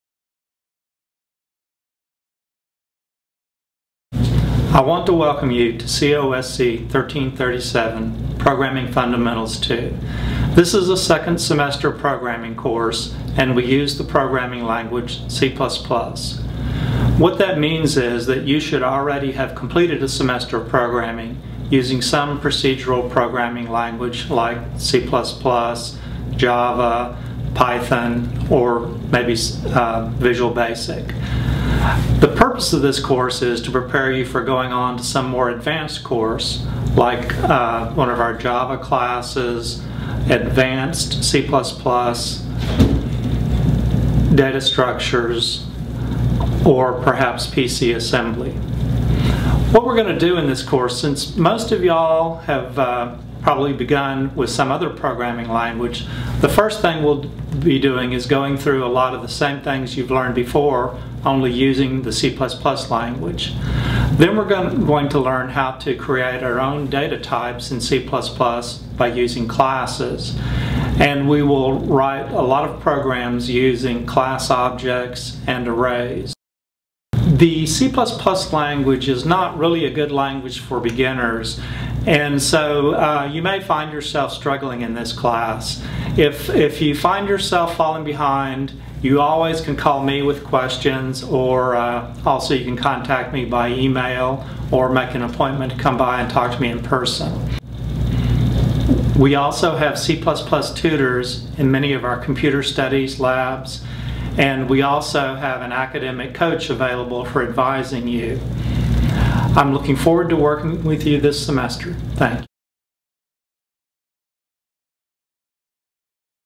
COSC 1337 Programming Fundamentals II — C++
A brief introduction of the course content, expectations and available support for this fundamental programming class.